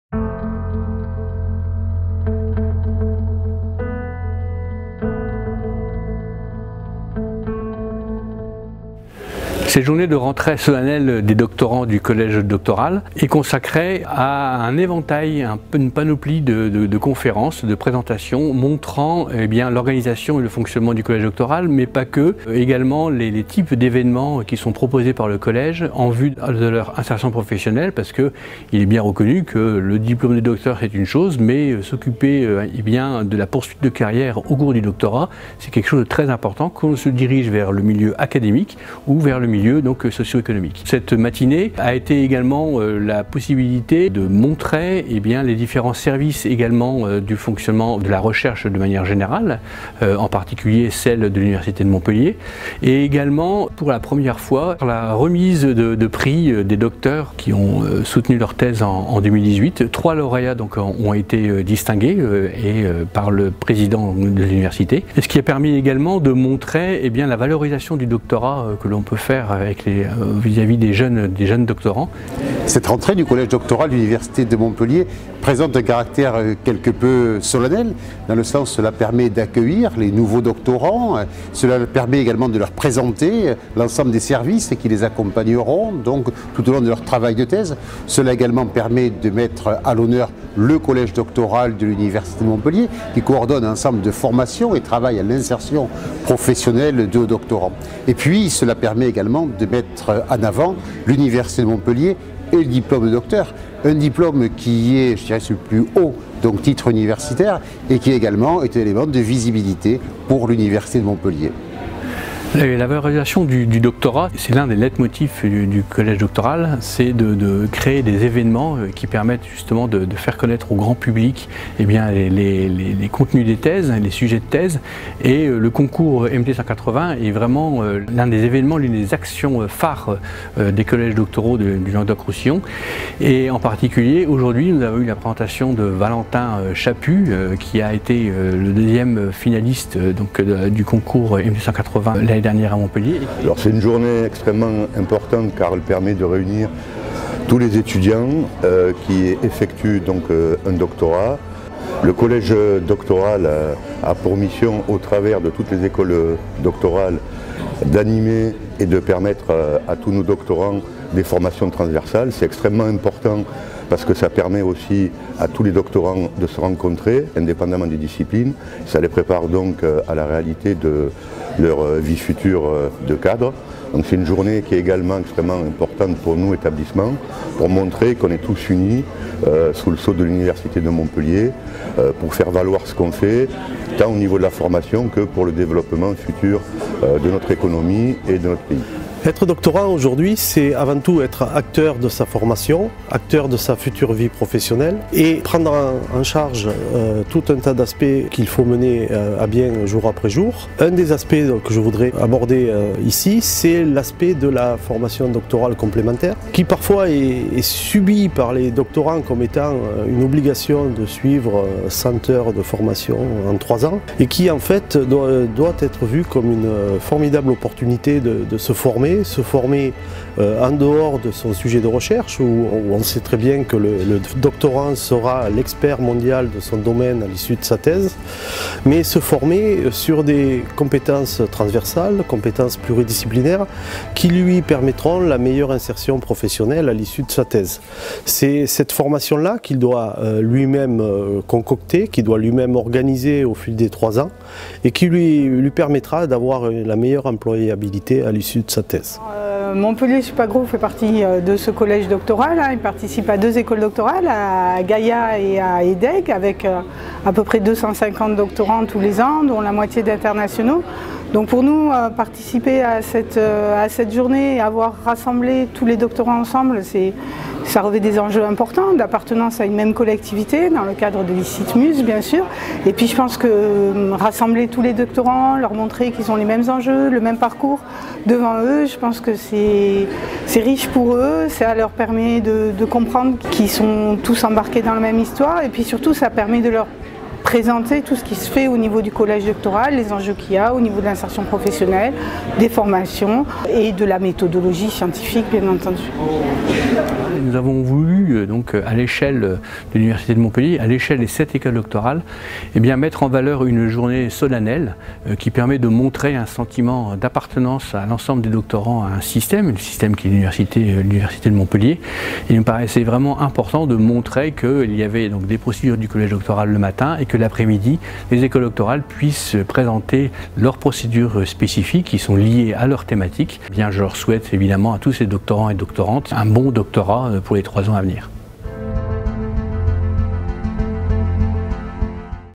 Collège Doctoral de l'Université de Montpellier : Rentrée Solennelle des Doctorants 2020
Le 16 janvier 2020 avait lieu la rentrée solennelle des doctorants organisée par le Collège Doctoral de l'Université de Montpellier. L'occasion de rassembler tous les nouveaux doctorants et de leur présenter non seulement les enjeux liés à leur travail de thèse mais également les formations et structures d'accompagnement qui, à leurs côtés, faciliteront l'aboutissement de leur projet de recherche.

Intervenants :
- Marc Bouvy, directeur du Collège Doctoral de l'Université de Montpellier
- Philippe Augé, Président de l'Université de Montpellier
- Pascal Dumy, directeur de l'ENSCM
- Pascal Nouet, directeur de l'école doctorale I2S
- Carole Sinfort, directrice déléguée de Montpellier SupAgro